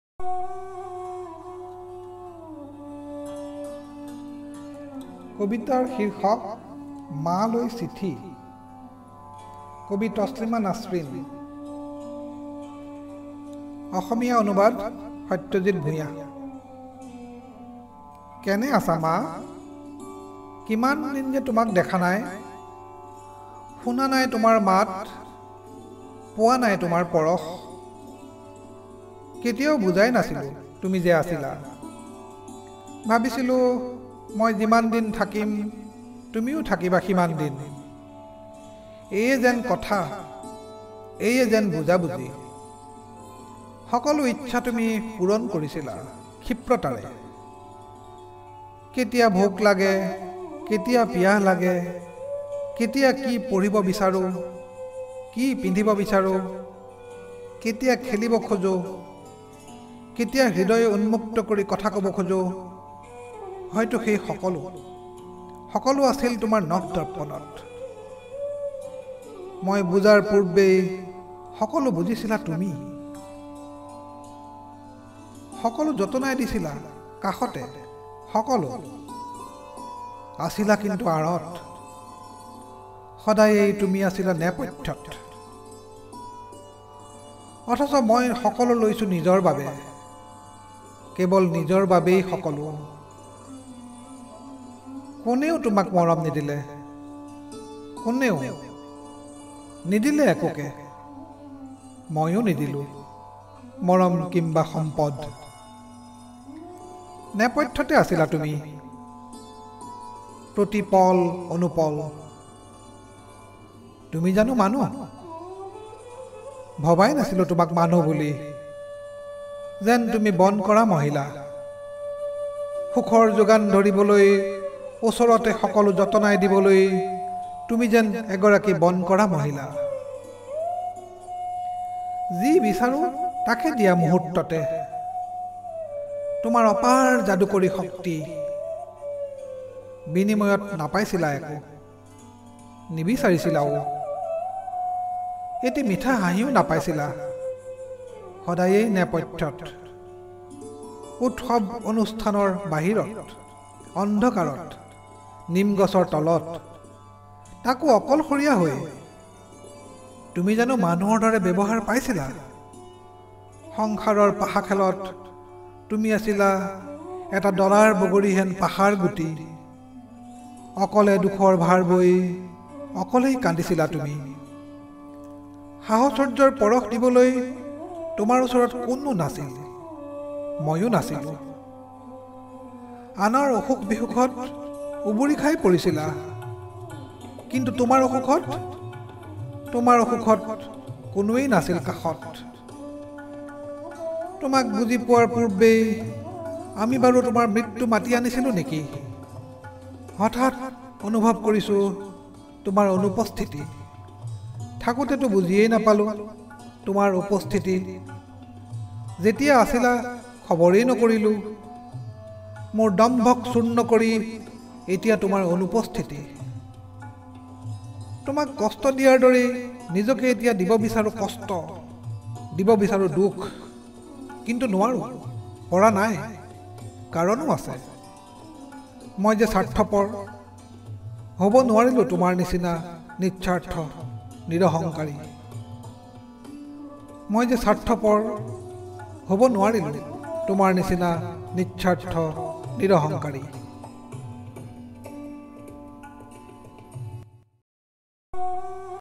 A letter to mother (Maa Loi Sithi)
A poem written by Taslima Nasrin and Translated & Recited by Dr Satyajit Bhuyan.